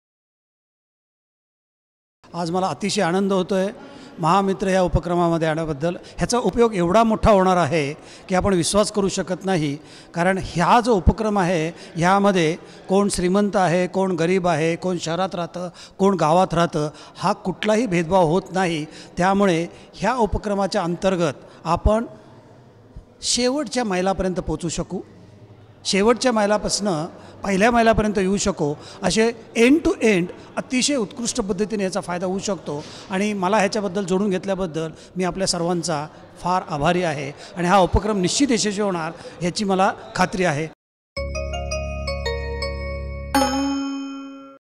सामाजिक कार्यकर्ते श्री. प्रदीप लोखंडे
महामित्र उपक्रमाच्या माध्यमातून कोणताही भेदभाव न होता शेवटच्या मैलापर्यंत पोहोचून, शेवटून पहिल्या मैलापर्यंत पोहोचता येईल- सामाजिक कार्यकर्ते श्री. प्रदीप लोखंडे